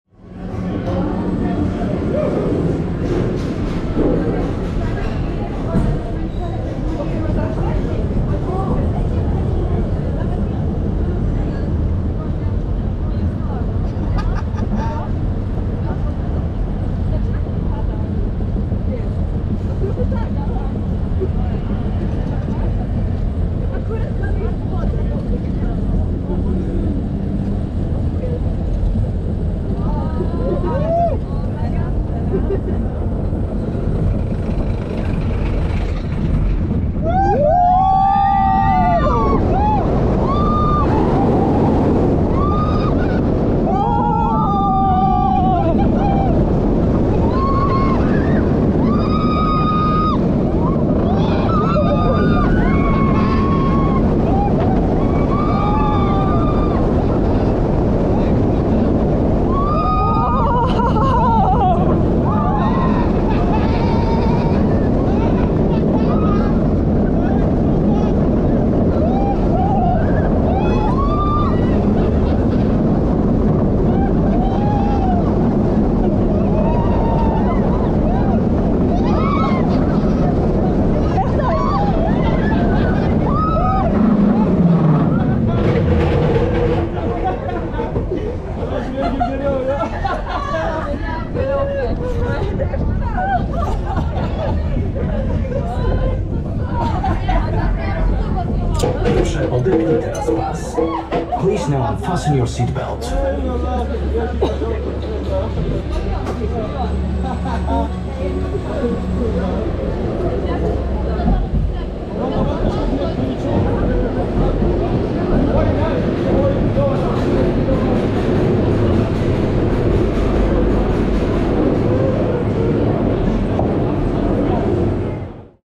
Hyperion /Energylandia [Onride]
Vidéo Onride  de Hyperion à Energylandia.

Constructeur: intamin AG
Longueur: 1 450,0 m
Hauteur: 77,0 m
Descente: 82,0 m
Vitesse: 142,0 km/h
Inversions: 1 (Dive drop)
Angle Vertical: 85,0°
G-Force: 4,8